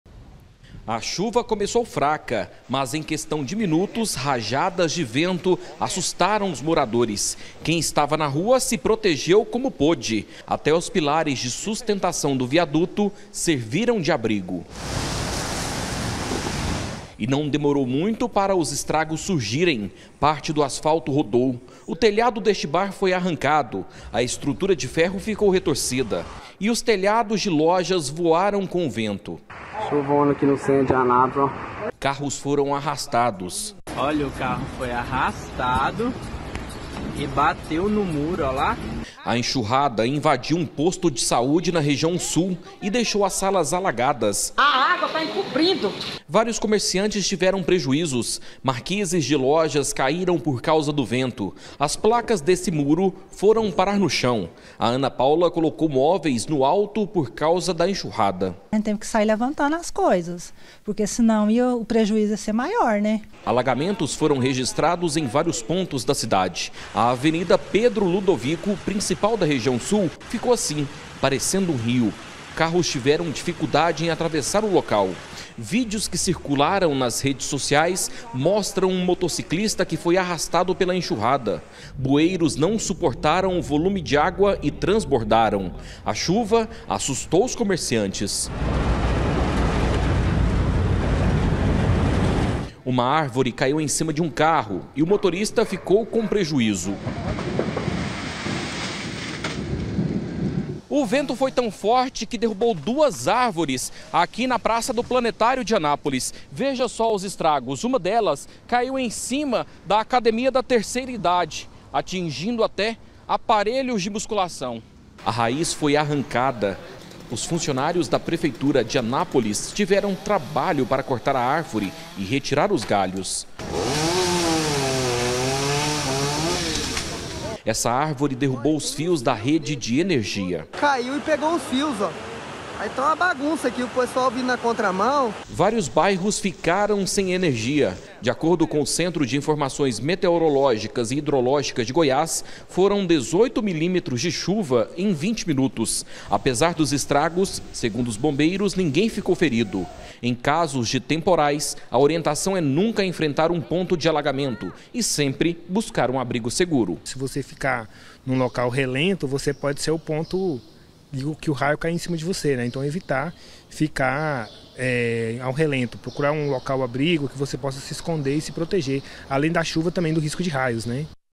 ANÁPOLIS: CHUVA FORTE ALAGA RUAS E DERRUBA ÁRVORES
UMA CHUVA RÁPIDA E FORTE PROVOCOU VÁRIOS ESTRAGOS, HOJE À TARDE EM ANÁPOLIS. UM POSTO DE SAÚDE FICOU ALAGADO.